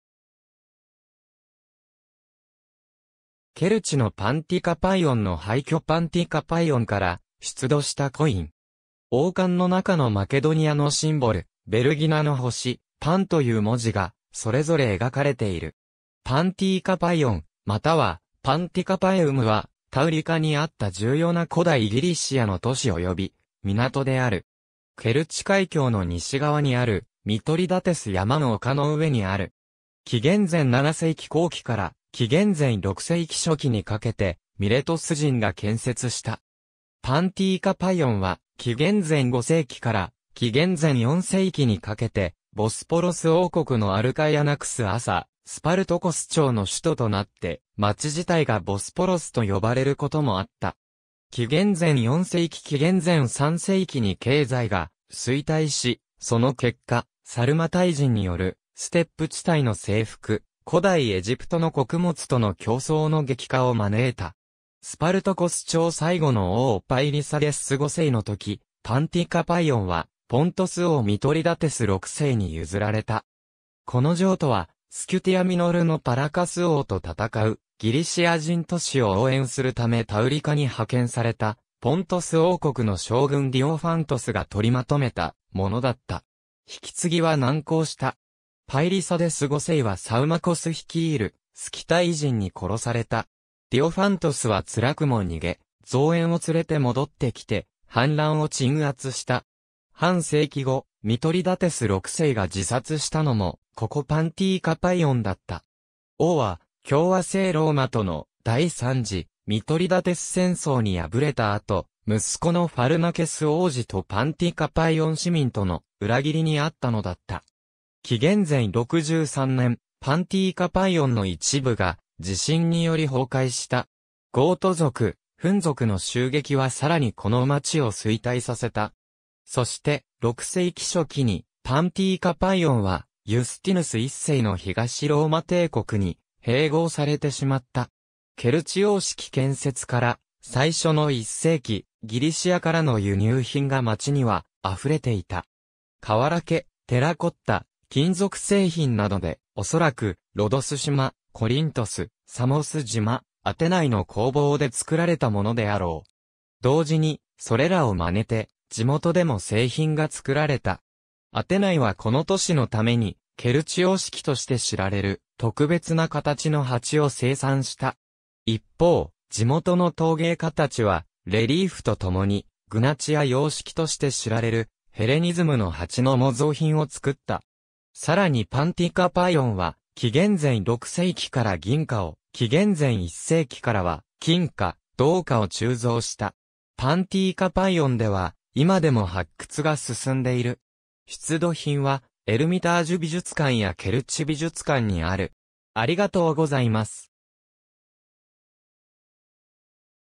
パンティカパイオン
ケルチのパンティカパイオンの廃墟 パンティカパイオンから出土したコイン。
（右）王冠の中のマケドニアのシンボル「ヴェルギナの星」、（左）PANという文字が、それぞれ描かれている。
パンティカパイオン（古代ギリシア語: Παντικάπαιον、Pantikapaion）または (現代ギリシア語転記:パンティカぺオン) パンティカパエウム（ラテン語: Panticapaeum）は、タウリカ（現クリミア自治共和国）にあった重要な古代ギリシアの都市および港である（現ケルチ）。
ケルチ海峡の西側にあるミトリダテス山の丘の上にある。
紀元前7世紀後期から紀元前6世紀初期にかけて、ミレトス人が建設した。
パンティカパイオンは、紀元前5世紀から紀元前4世紀にかけて、ボスポロス王国のアルカイアナクス朝、スパルトコス朝（ともに王はギリシア人）の首都となって、町自体がボスポロスと呼ばれることもあった。
紀元前4世紀〜紀元前3世紀に経済が衰退し、その結果、サルマタイ人によるステップ地帯の征服、古代エジプトの穀物との競争の激化を招いた。
スパルトコス朝最後の王パイリサデス5世の時、パンティカパイオンはポントス王ミトリダテス6世に譲られた。
この譲渡は、スキュティア・ミノルのパラカス王と戦うギリシア人都市を応援するためタウリカに派遣された、ポントス王国の将軍ディオファントスが取りまとめたものだった。
引き継ぎは難航した。
パイリサデス5世はサウマコス率いるスキタイ人に殺された。
ディオファントスは辛くも逃げ、増援を連れて戻ってきて、反乱を鎮圧した（紀元前110年頃）。
半世紀後、ミトリダテス6世が自殺したのも、ここパンティカパイオンだった。
王は共和政ローマとの第三次ミトリダテス戦争に敗れた後、息子のファルナケス王子とパンティカパイオン市民との裏切りにあったのだった。
紀元前63年、パンティカパイオンの一部が地震により崩壊した。
ゴート族、フン族の襲撃はさらにこの町を衰退させた。
そして、6世紀初期に、パンティカパイオンはユスティヌス1世の東ローマ帝国に併合されてしまった。
ケルチ様式 建設から最初の1世紀、ギリシアからの輸入品が町には溢れていた。
土器 、テラコッタ、金属製品などで、おそらくロドス島、コリントス、サモス島、アテナイの工房で作られたものであろう。
同時に、それらを真似て、地元でも製品が作られた。
アテナイはこの都市のために、ケルチ様式として知られる、特別な形の鉢を生産した。
一方、地元の陶芸家たちはレリーフとともに、グナチア様式として知られるヘレニズムの鉢の模造品を作った。
さらにパンティカパイオンは、紀元前6世紀から銀貨を、紀元前1世紀からは金貨・銅貨を鋳造した。
パンティカパイオンでは今でも発掘が進んでいる。
出土品はエルミタージュ美術館やケルチ美術館にある。